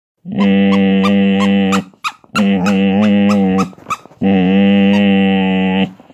Malinois Can Talk
certainly to my knowledge, one of the most vocal dogs you'll ever know! you can literally have a back and fourth conversation with Radek!! I only wish I knew what he was saying.